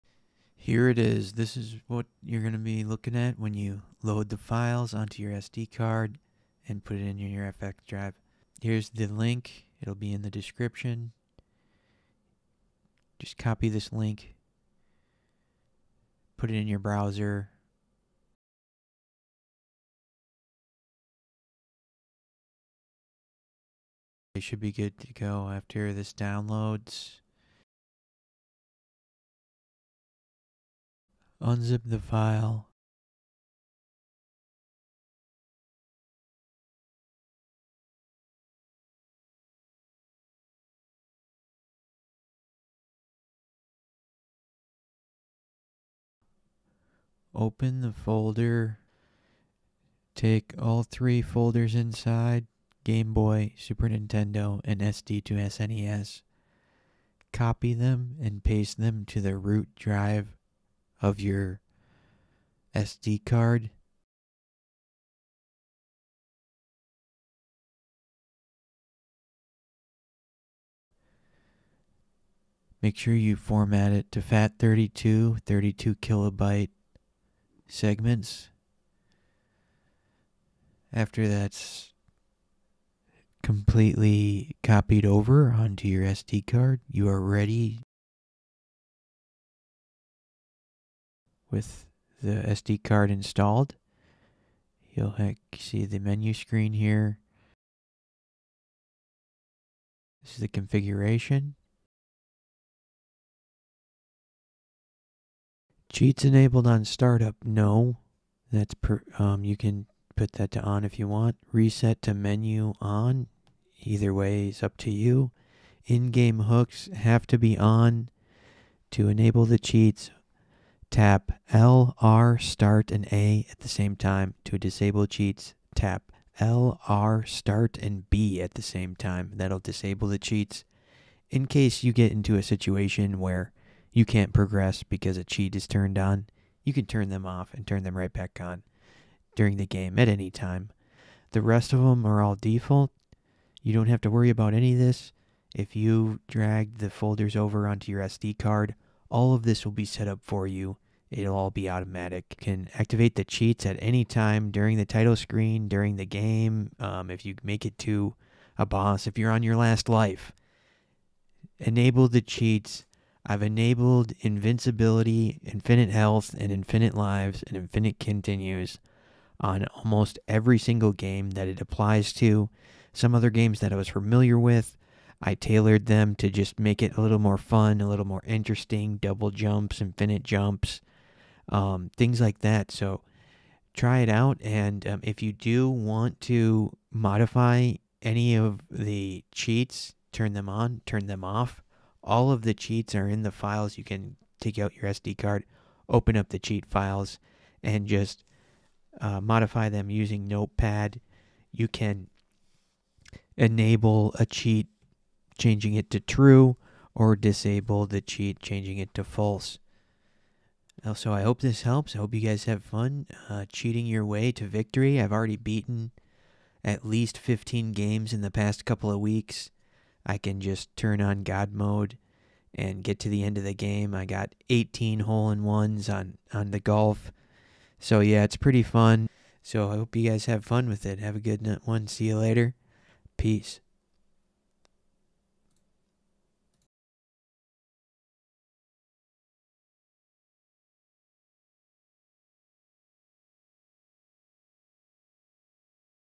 FX PAK PRO SETUP[DOWNLOAD-EXTRACT-PLAY]SAVESTATES AND CHEATS WORK TOGETHER. NEW UPDATE
Alternative Link

SAVESTATES AND CHEATS WORK TOGETHER FLAWLESSLY!

THE IN GAME BUTTONS FOR GAME GENIE AND SAVE STATES
Turn on cheats L R A START
Turn off cheats L R B START
Save state R SELECT
Load state L SELECT

IF YOU WANT TO SAVE MULTIPLE STATES YOU CAN CHOOSE UP TO 4 SLOTS WHILE IN GAME

SLOT 1: D-PAD UP SELECT AND START
SLOT 2: D-PAD RIGHT SELECT AND START
SLOT 3: D-PAD DOWN SELECT AND START
SLOT 4 D-PAD LEFT SELECT AND START

ALL OF THIS IS CUSTOMIZABLE IN file location
sd2snes/config.yml

All fixed
All Game boy
All SNES
All Cheats
All save states